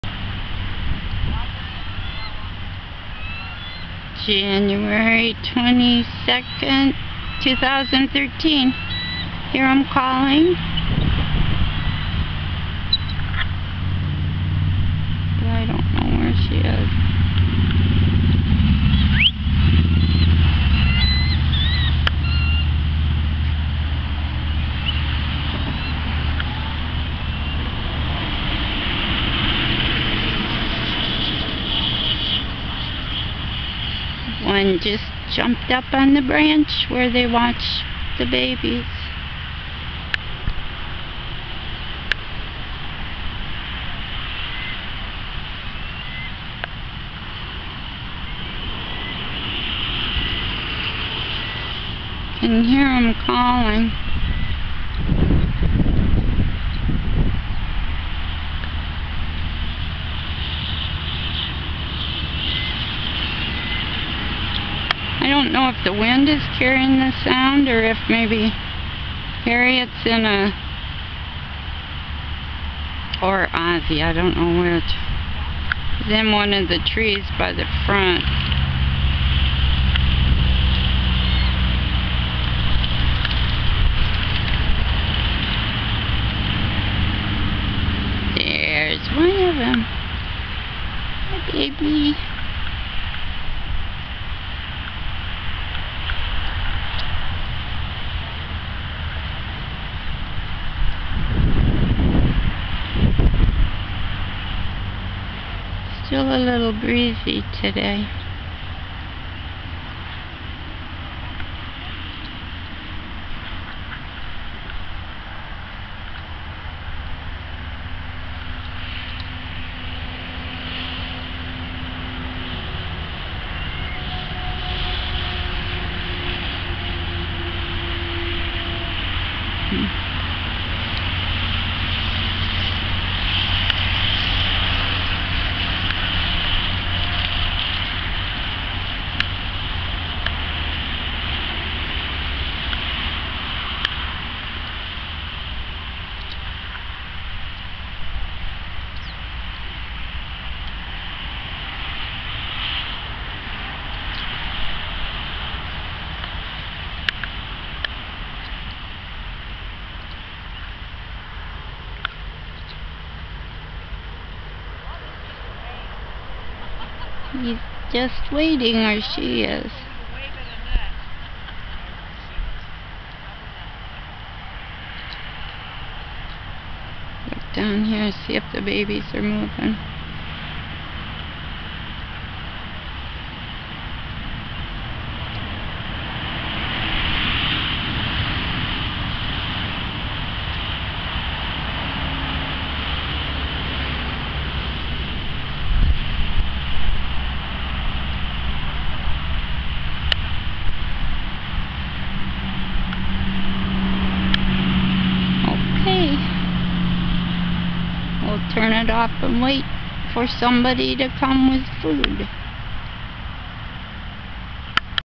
SW Florida Eagles_Ozzie And Harriet Enjoy Quiet Time Part I 1-22-13
Harriet calls from the nest and launches to the "attic". While filming I could not see that Ozzie was well hidden right above Harriet. Their calls to each other continue throughout the video. Thank you Dick Pritchett Real Estate for maintaining such a suitable tree and environment for our Bald Eagle Family.